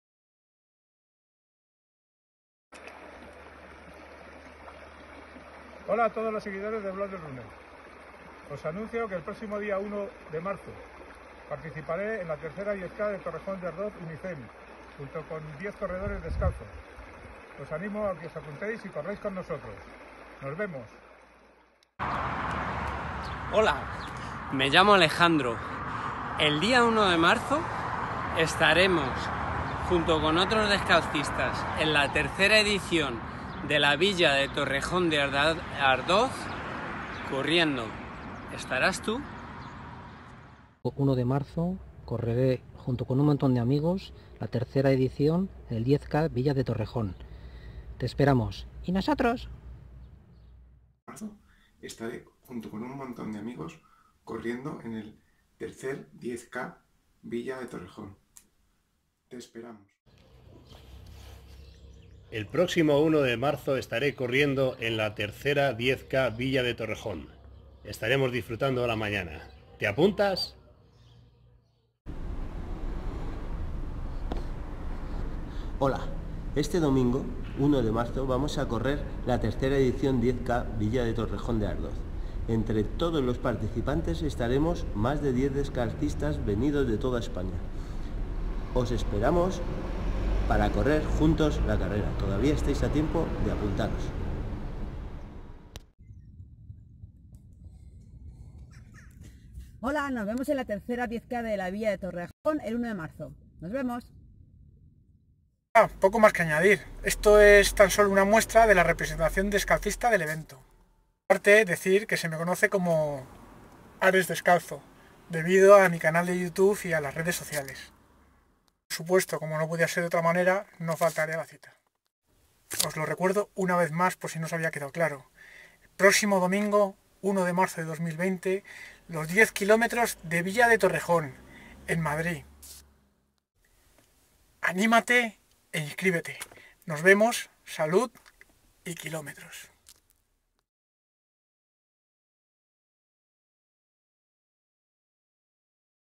Vídeo promoción descalcista 10K Villa de Torrejón 2020
Vídeo promocional de los 11 corredores descalzos de la edición III de Villa de Torrejón